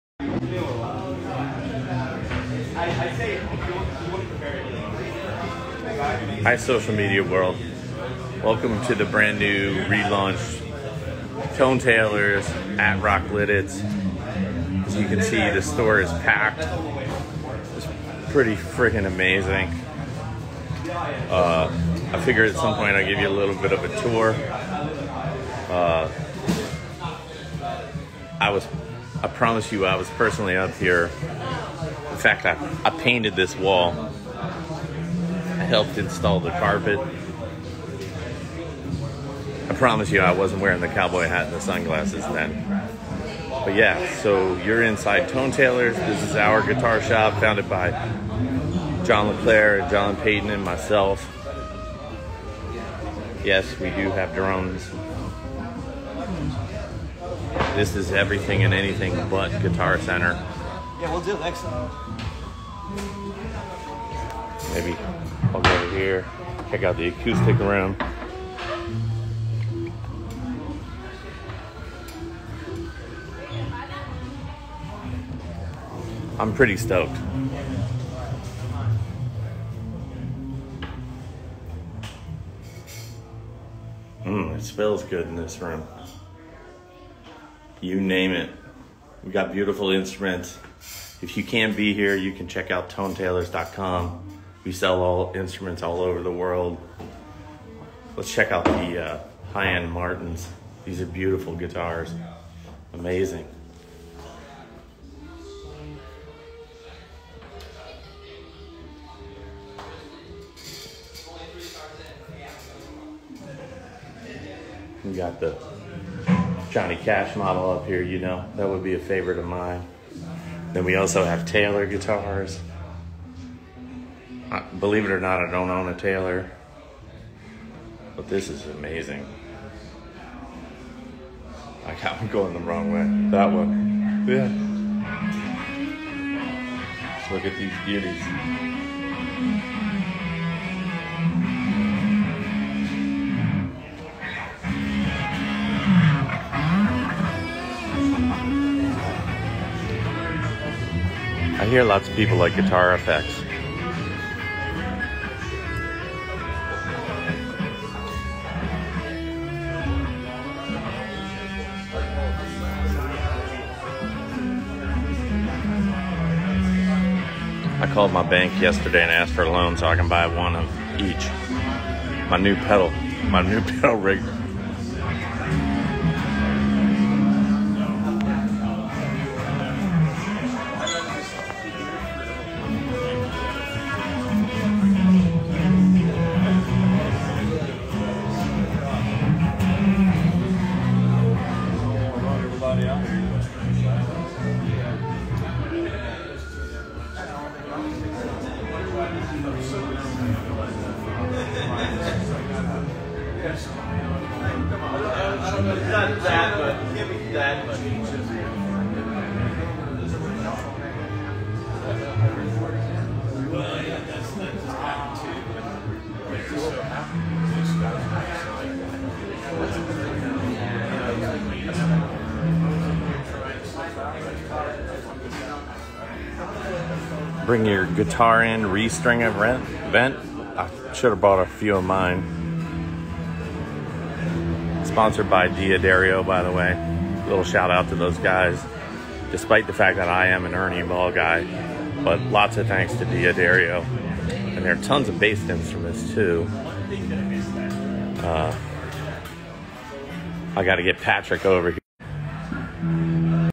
Chad Taylor at reopening of Tone Tailors Instruments Store - 15 May 2022
Chad Taylor on Instagram giving a tour of the reopening of Tone Tailors Instruments store in PA.

Tone Tailors - Guitars, Bass, Drums, Pro Audio and Lessons